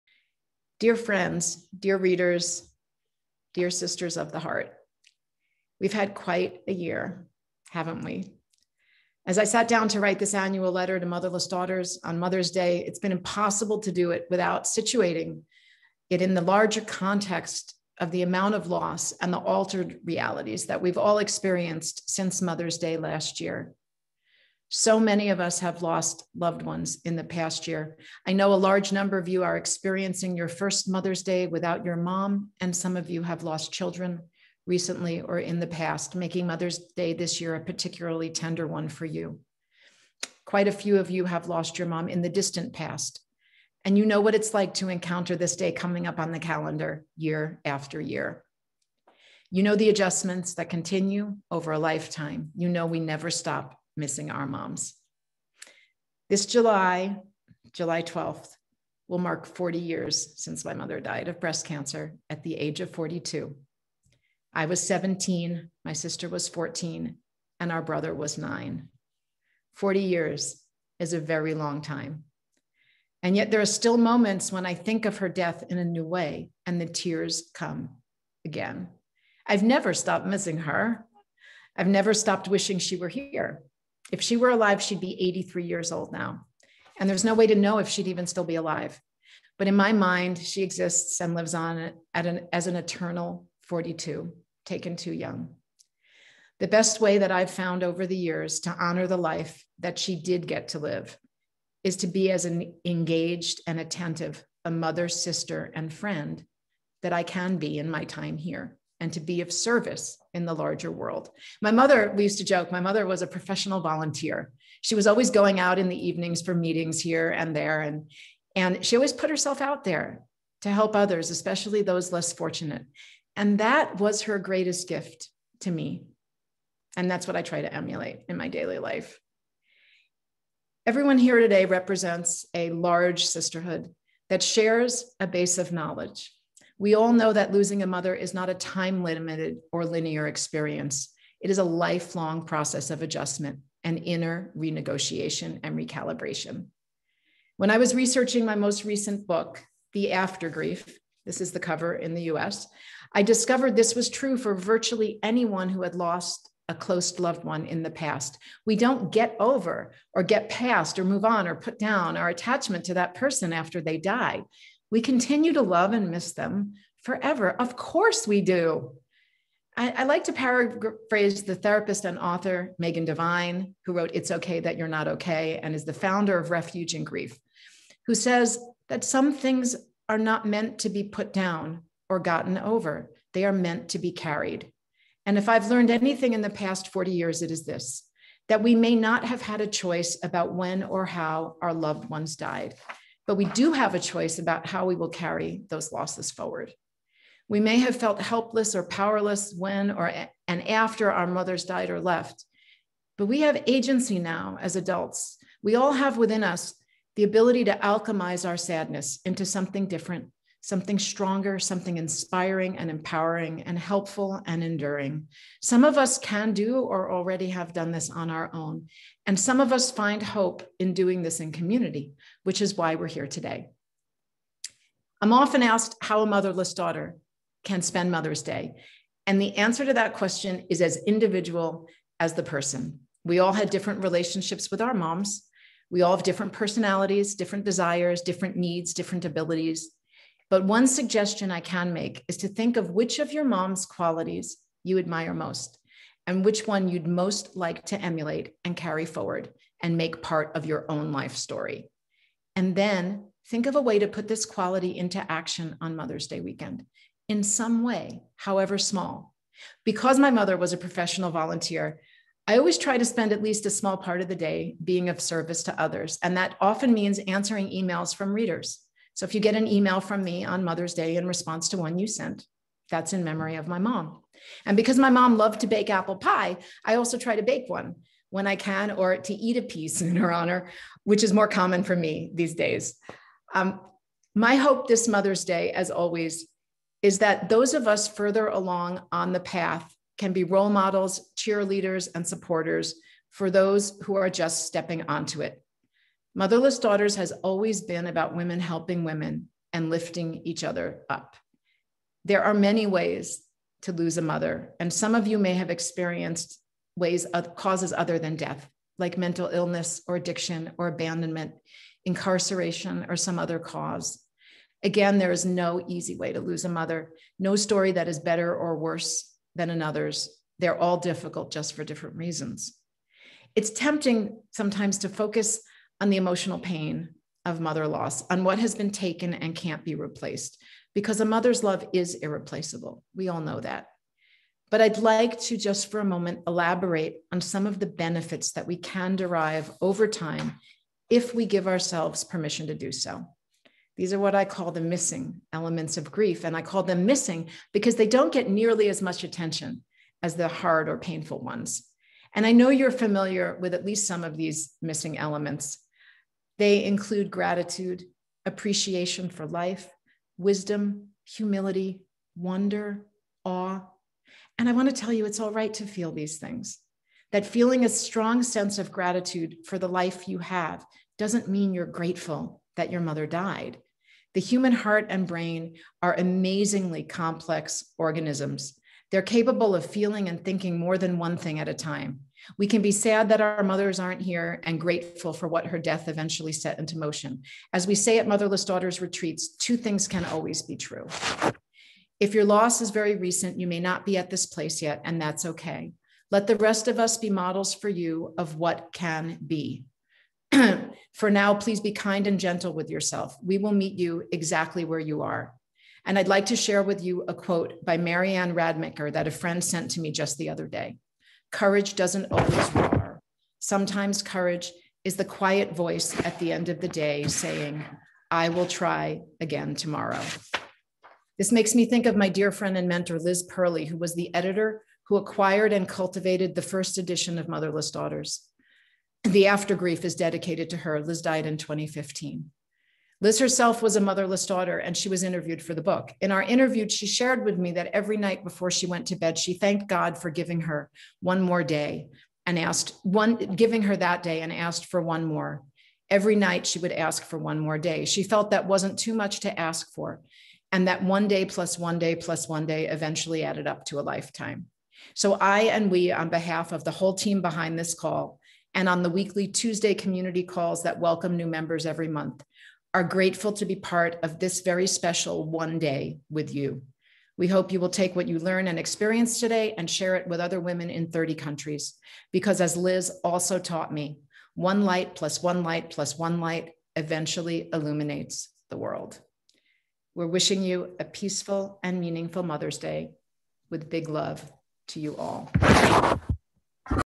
Hope Edelman's Annual Open Letter to Motherless Daughters
Hope Edelman's Annual Open Letter to Motherless Daughters originally read on May 8th, 2021 at the very first Worldwide Day of Remembrance